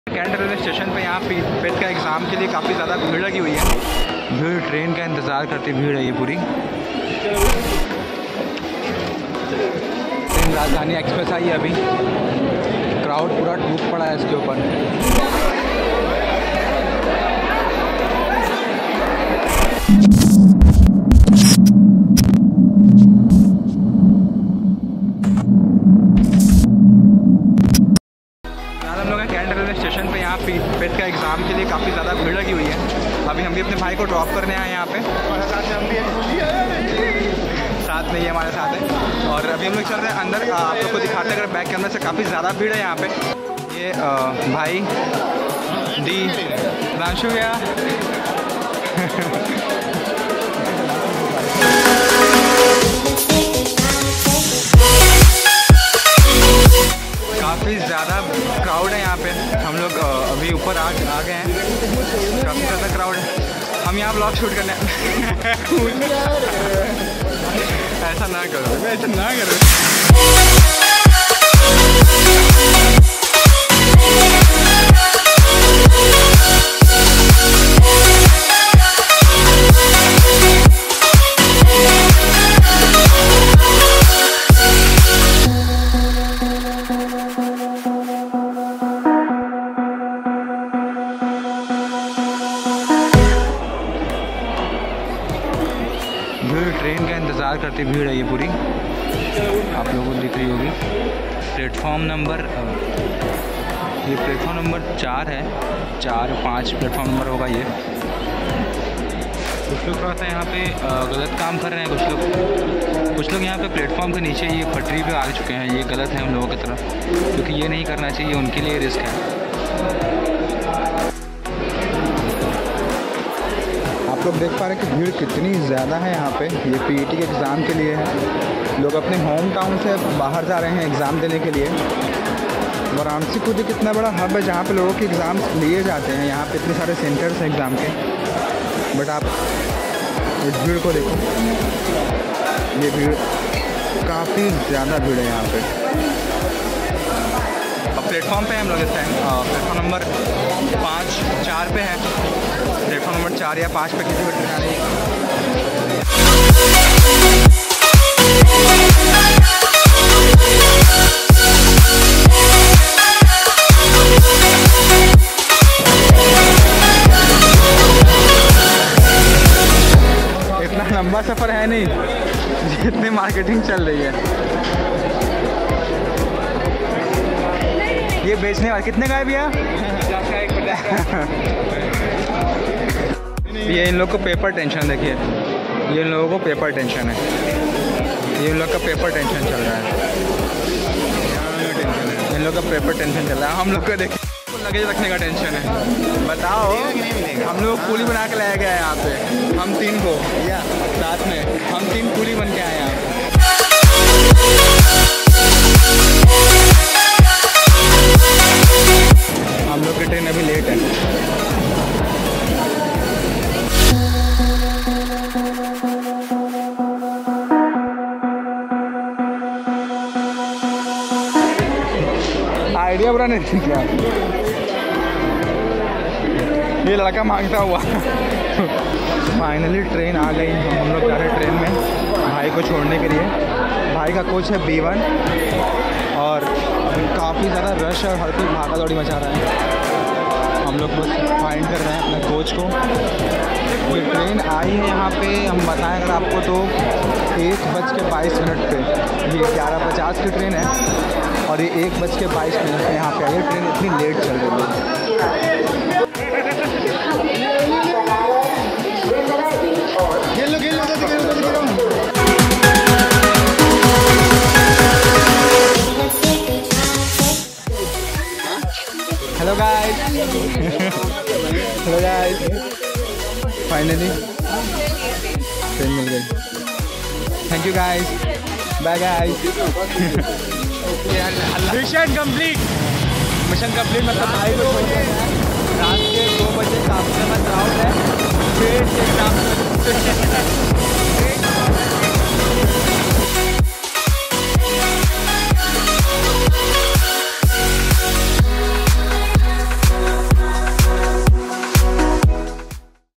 UPSSSC PET Exam heavy crowd || Varanasi || 2022
PET exam ke time varanasi cantt railway station ki bheed .